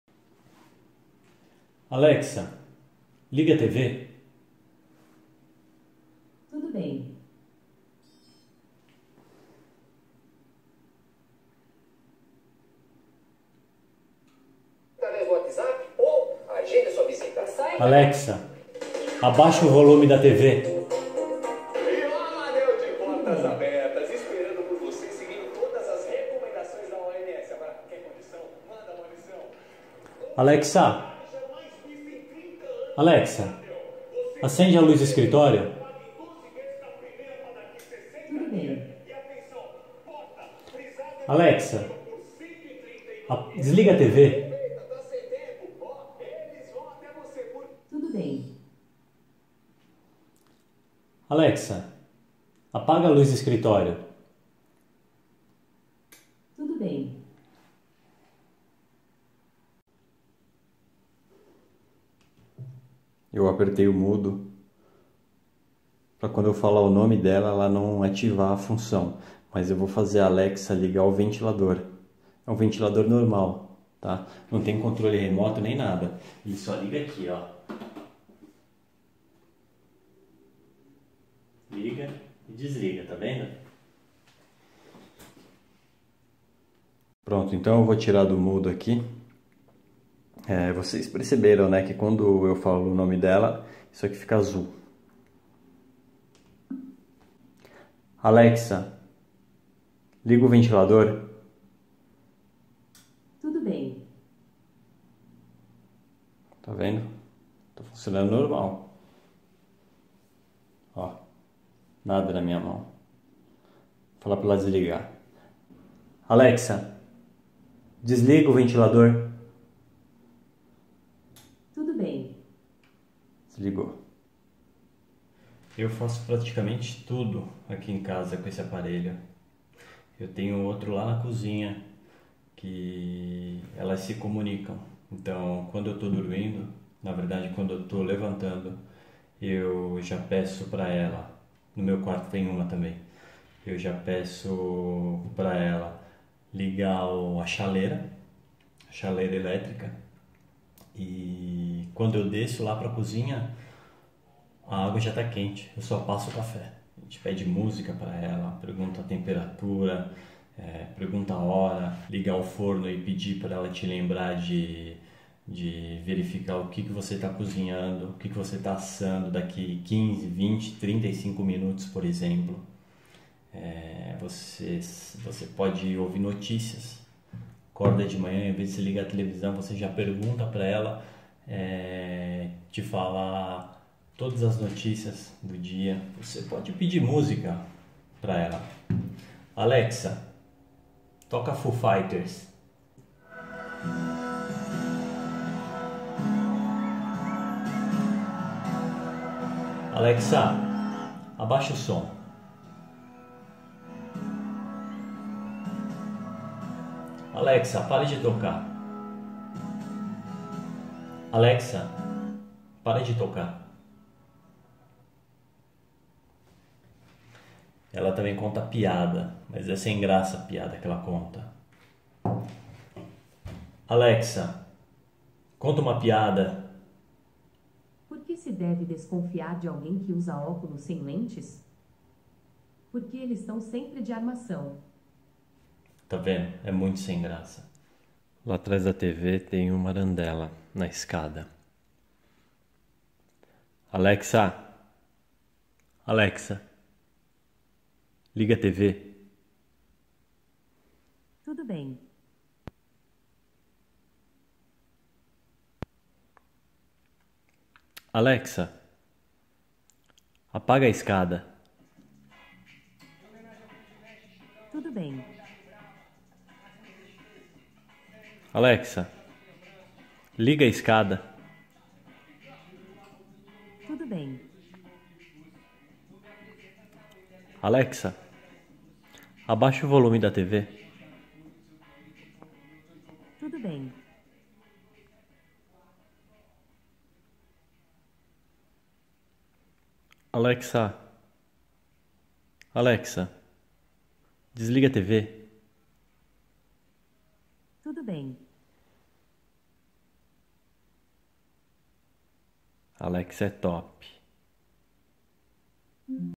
MINHA CASA DO FUTURO - ALEXA ECHO DOT 3ª GERAÇÃO
O que você pode fazer com o Echo Dot 3ª Geração na sua casa!
"Alexa...ascenda as luzes da sua casa", "ligue o ar condicionado", "ventilador", "tv", "máquina de café" e muito mais!